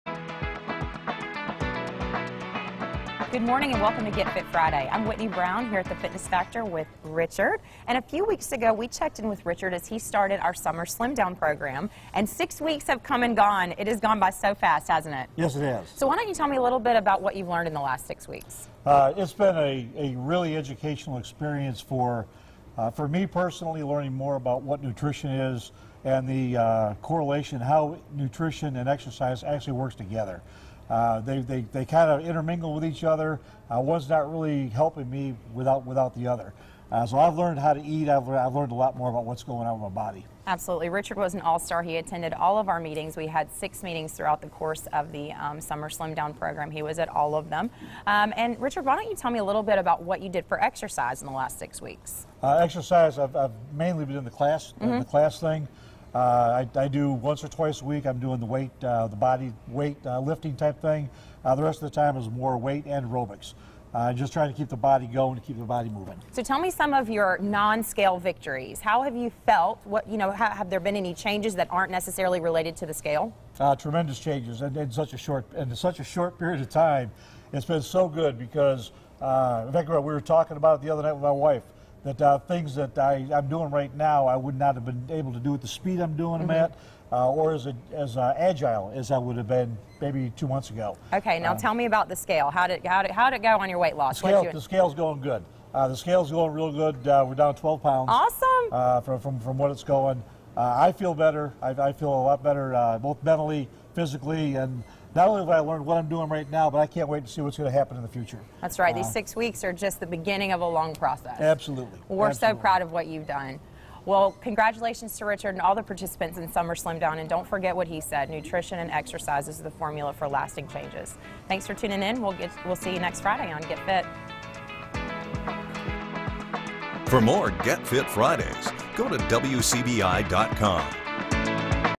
Get Fit Friday 6/8/18 - Summer Slim Down
Six weeks ago, Richard took up the challenge for the Summer Slim Down at The Fitness Factor.  Today, Whitney Brown finds out how much progress he's made over the past several weeks using a combination of diet and exercise.